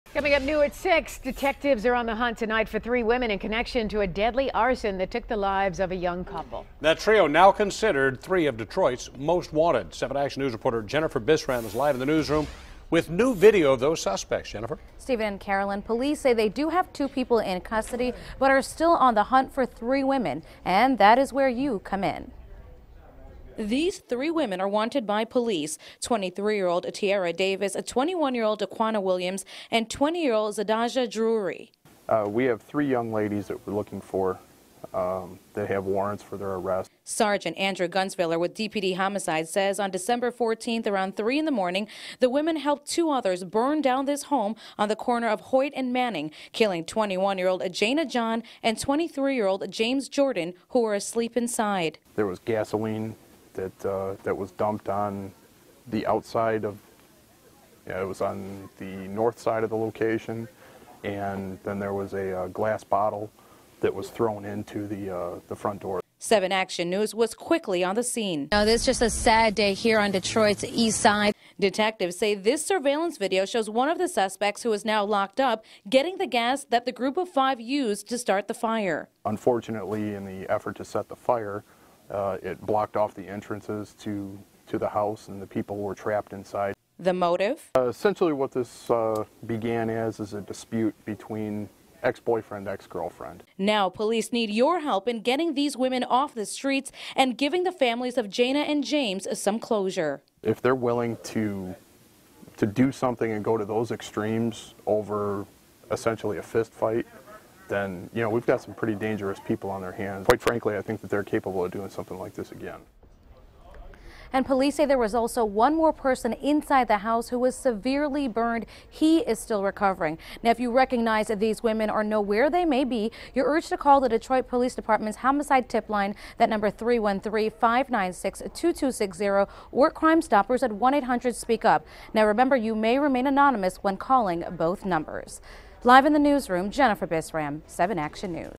Arson suspects are Detroit's Most Wanted
Three women who was suspects in a deadly arson are the newest members of Detroit's Most Wanted.